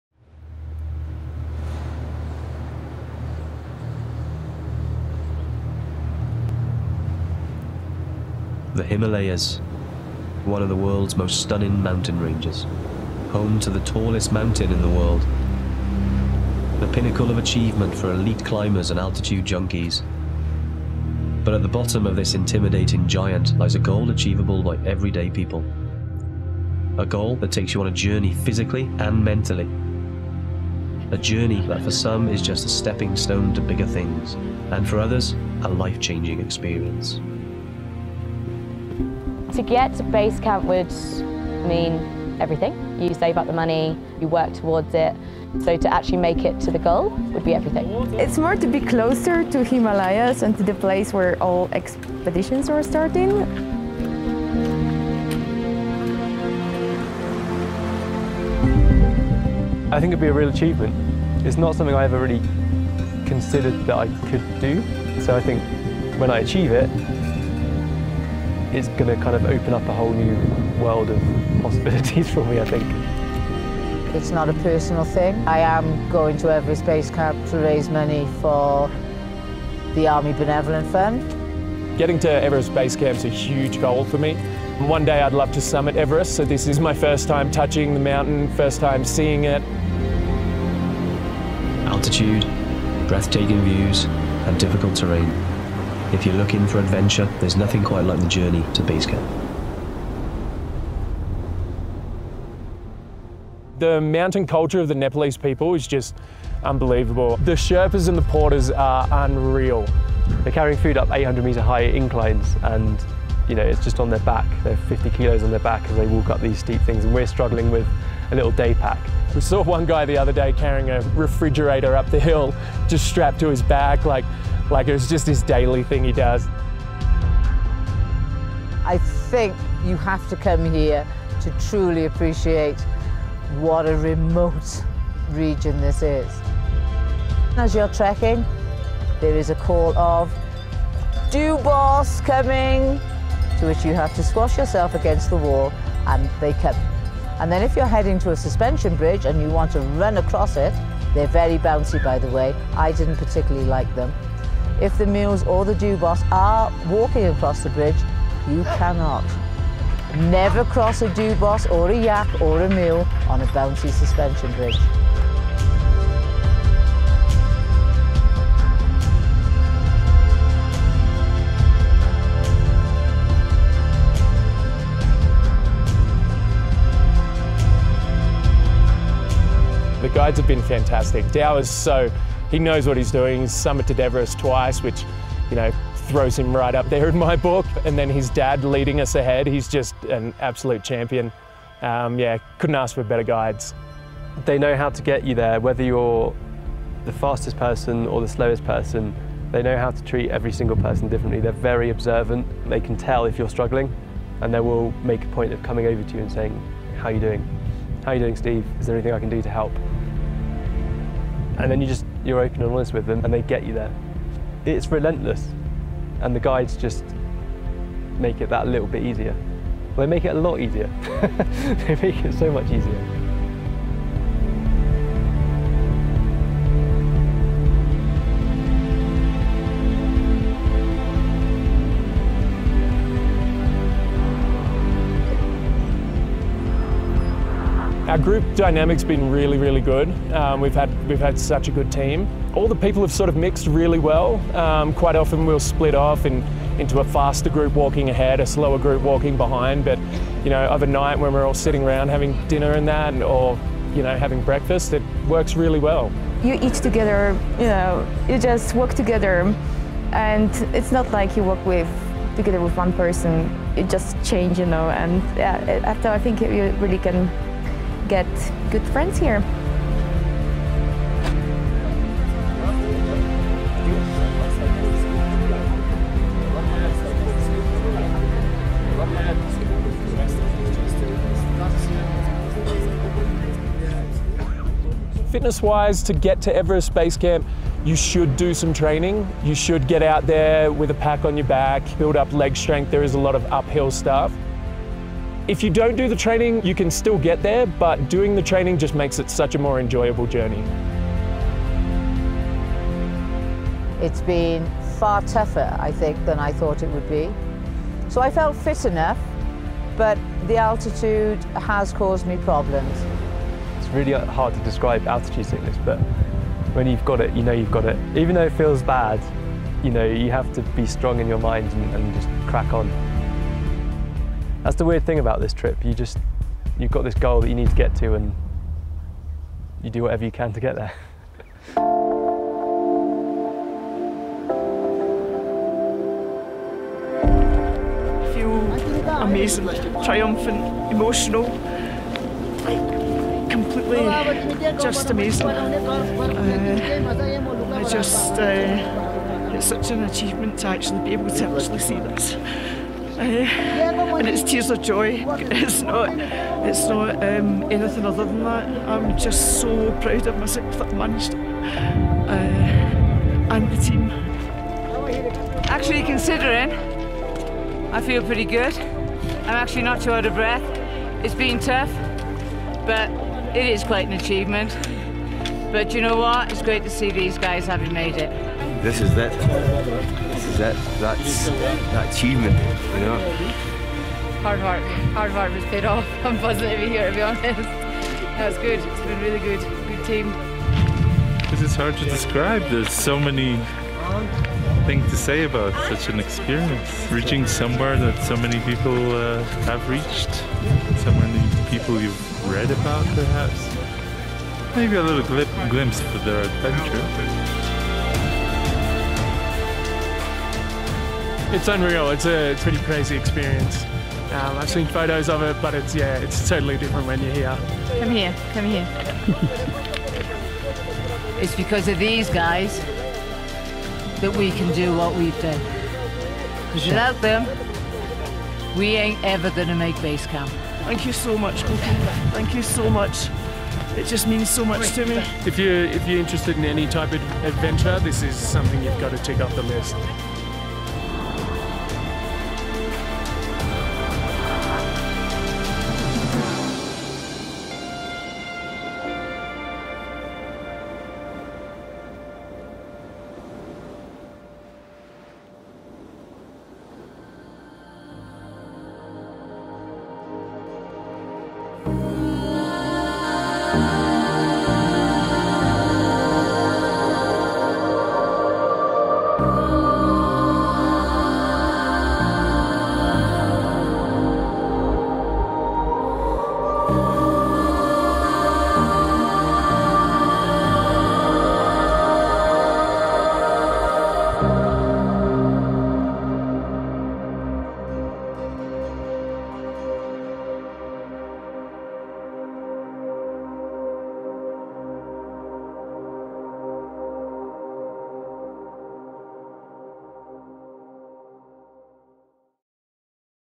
Mount Everest Base Camp - A short documentary
The Himalayas - one of the most beautiful mountain ranges in the world. Home to Mount Everest, the tallest mountain in the world. The pinnacle of achievement for elite mountain climbers and altitude junkies. But at the bottom of this intimidating giant lies a goal achieveable by every day people - Mount Everest Base Camp.

This short documentary follows a small group of adventurers on their journey from Lukla in the Himalayas to Everest Basecamp, at an altitude of 5,380 meters.

It looks at the expereinces one might encounter, the fun times, potential difficulties and feeling of achievement on reaching the destination.

Filmed on a Panasonic GH5 and a DJI OSMO by Steve Bollschweiler.